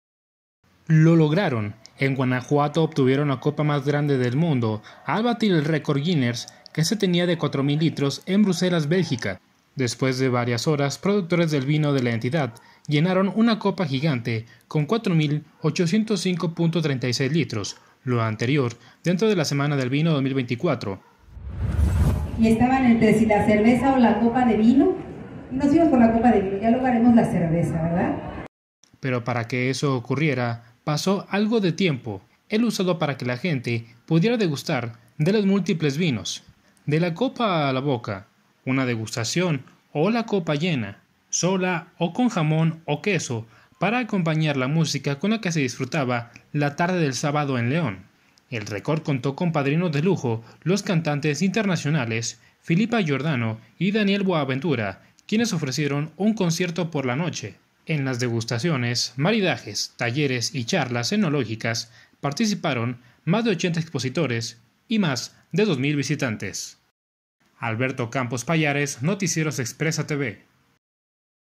Guanajuato logra récord Guiness por copa de vino más grande
Sigue las Noticias

Únete a la comunidad de WhatsApp y recibe las ultimas noticias